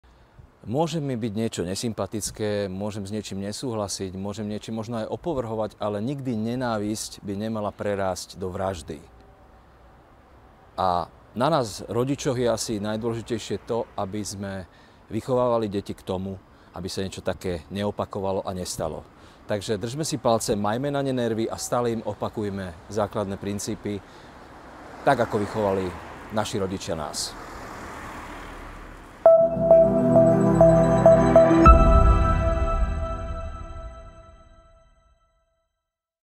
Vladimír Kobielsky | Nie je nám to jedno 💔 smesvami 🏳️‍🌈
Kde sa v ľuďoch berie toľko nenávisti? 🙁 Herec Vlado Kobielsky apeluje v tejto neľahkej dobe najmä na rodičov 🙏 Aj on sa v spolupráci s TV Markíza rozhodol vyjadriť podporu LGBTI+ komunite 🏳️‍🌈

Nie je nám to jedno 💔 Známe osobnosti sa rozhodli, že nebudú mlčať a dali v spolupráci s nami jasne najavo svoj názor na udalosti posledných dní. Postupne zverejníme ich silné odkazy plné podpory. Sme s vami 🏳️‍🌈